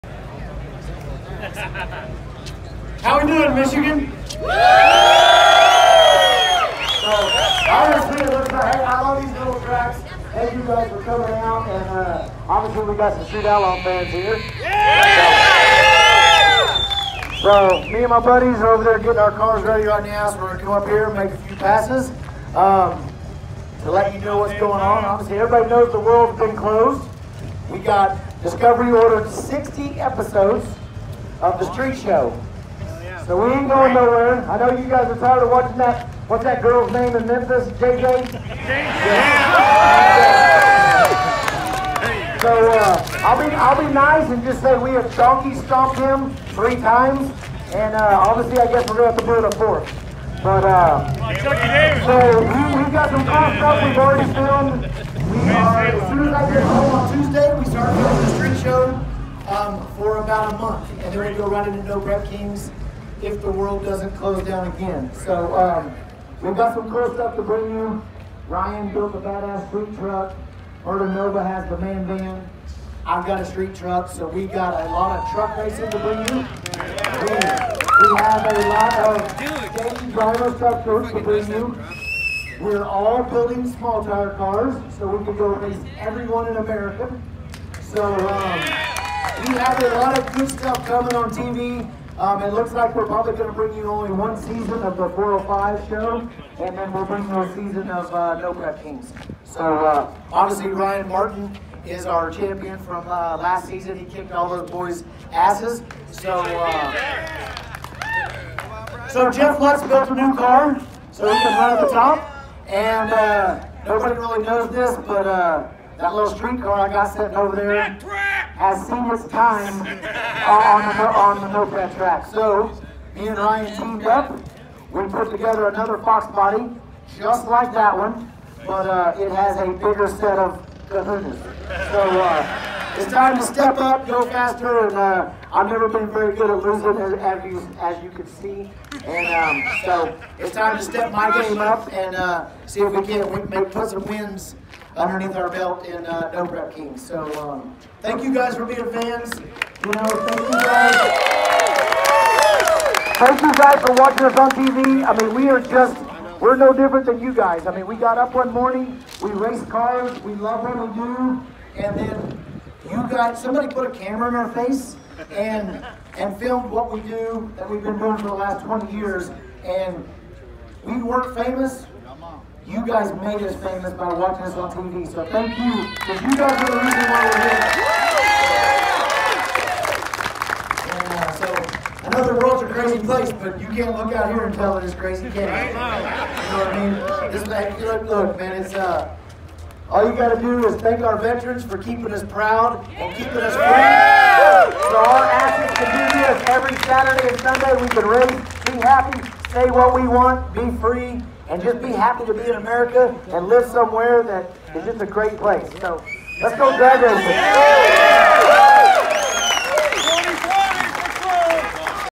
Chuck Seitsinger addressing the fans!
Chuck Seitsinger at Lapeer international Raceway